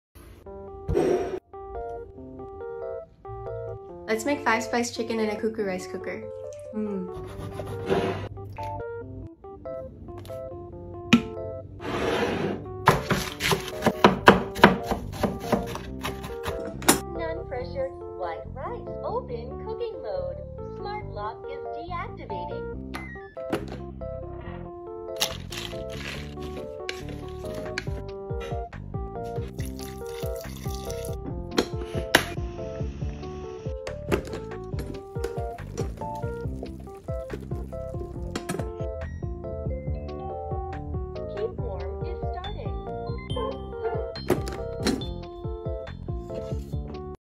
Joanna Chung's Five Spice Chicken in a CUCKOO Rice Cooker
Follow Joanna!